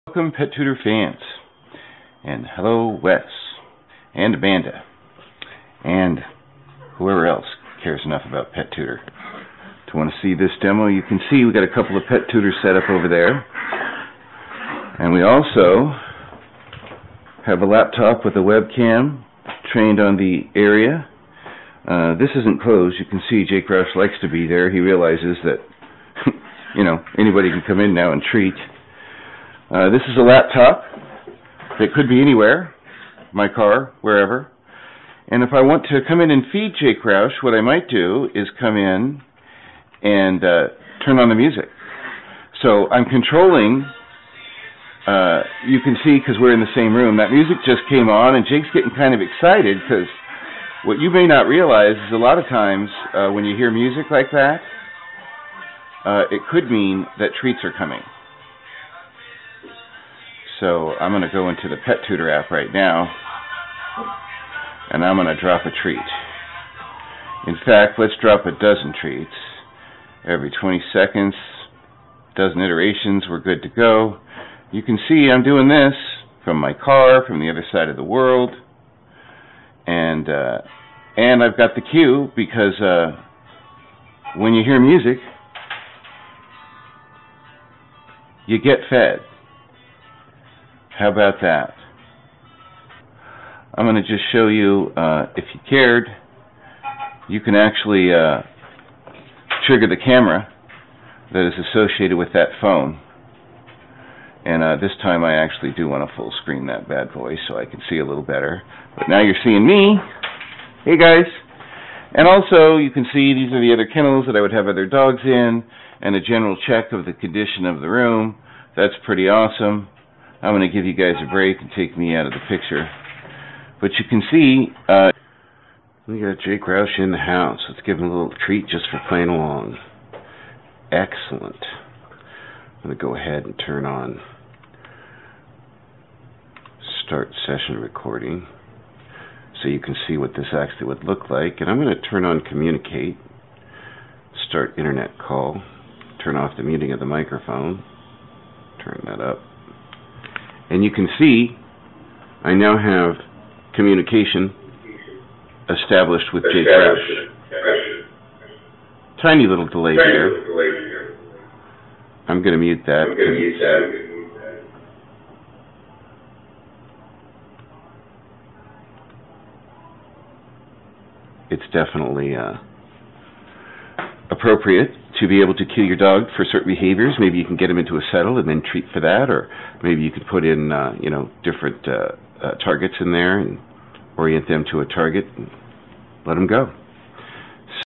Feeding with PT Remotely
This illustrates how I can utilize Pet Tutor, a laptop with WebCam, a Smart Phone, and TeamViewer feed any schedule anytime from anywhere.